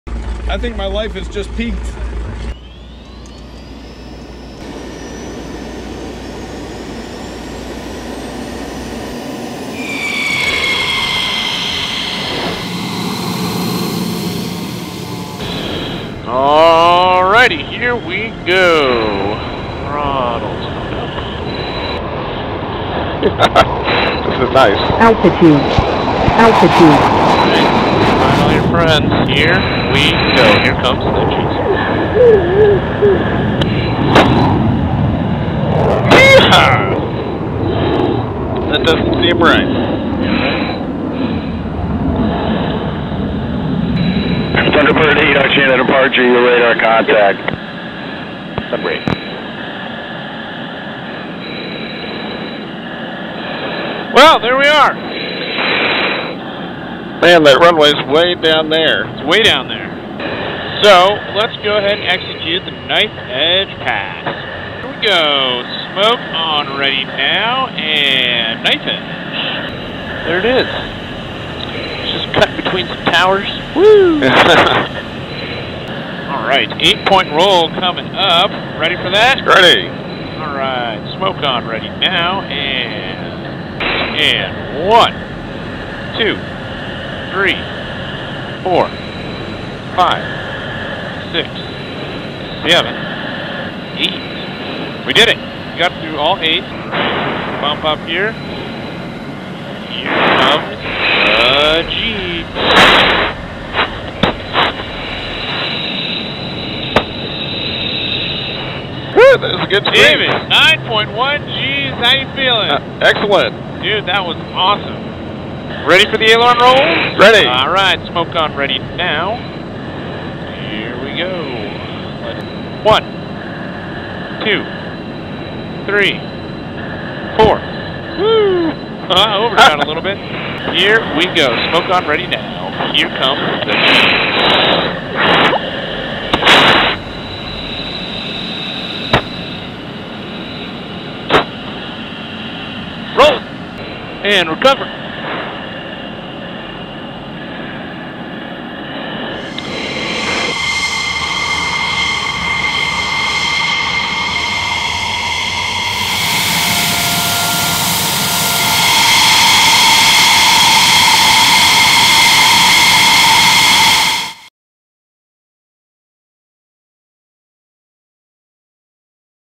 Flying an F16!
What's it like to fly an F16? AOPA Pilot editor at large Dave Hirschman takes the ride of his life in a US Air Force Thunderbird F16 Viper, pulls 9 g's, and flies some of the incredible aerobatic maneuvers the Thunderbirds perform during an airshow.